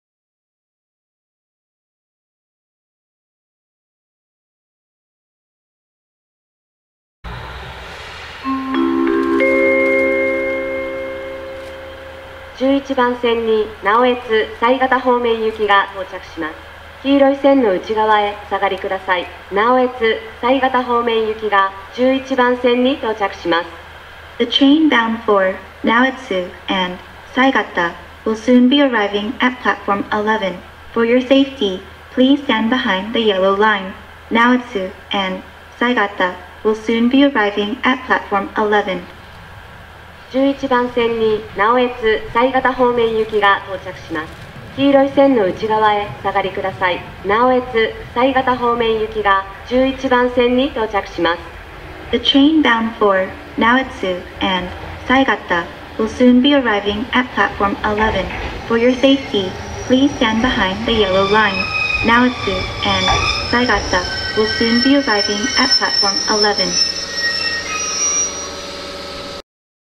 【駅放送】北越急行十日町駅11番線接近放送
2022年11月23日(水・祝)収録。
北越急行十日町駅11番線(直江津・犀潟方面行)の接近放送です。女性の声の放送の後に、英語放送が流れます。列車が止まる寸前まで、放送が繰り返されます。飯山線ホームに泊まっているディーゼル車のアイドリング音が入っていますが、ご了承ください。
直江津D51レールパークに向かう途中で寄りました。鉄印帳に記帳し、超快速に乗りましたが、本当に速くて快適でした。

接近列車：十日町9:41発超快速スノーラビット新井行き(越後湯沢始発、HK100形2両ワンマン)